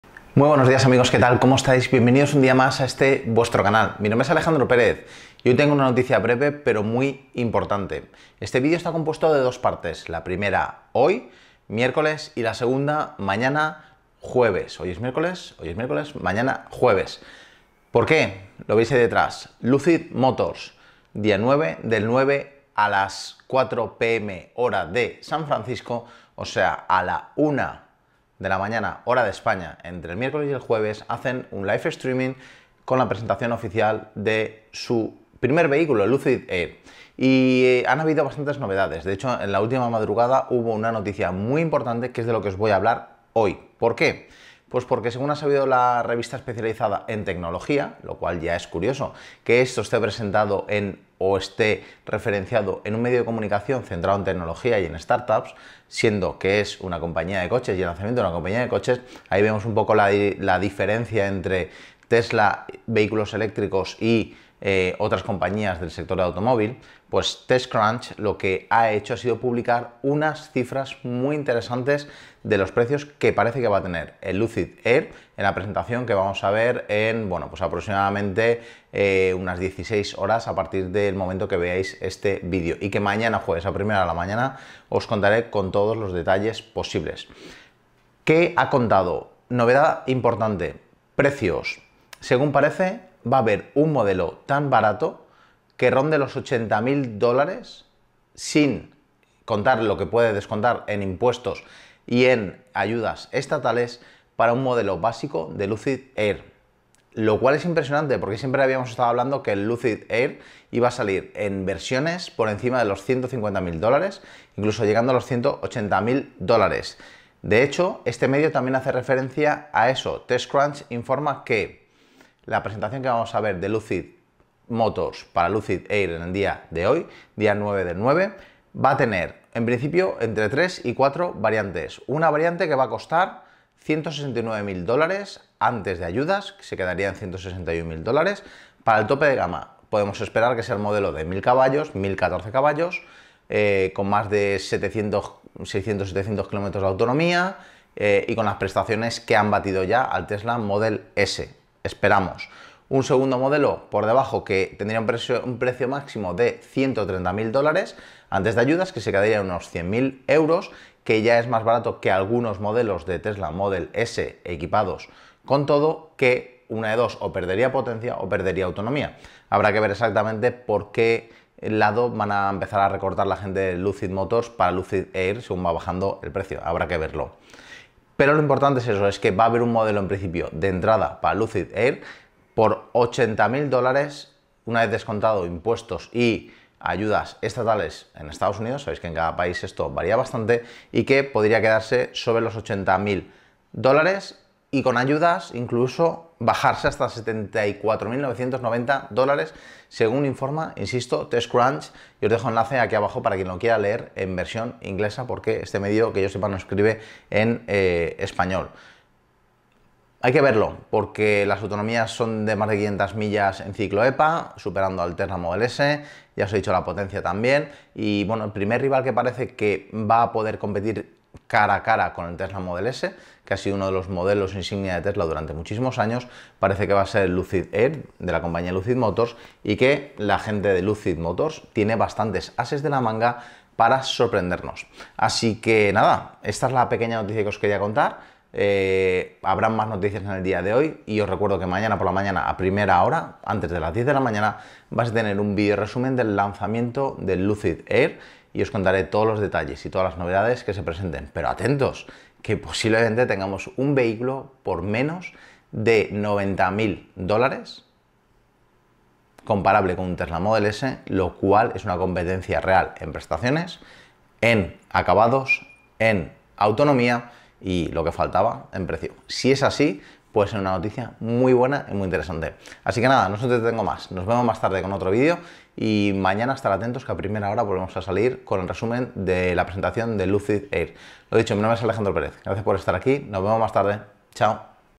LUCID AIR: Más barato que un Tesla Model S? 🤯🤯
Lucid Air tiene previsto anunciar todos los detalles de su nuevo coche eléctrico, el Lucid Air hoy y la novedad llega porque TechCrunch anunció ayer los precios del Lucid Air que mejoran o por lo menos se quedan casi iguales en su modelo básico con respecto a los Tesla.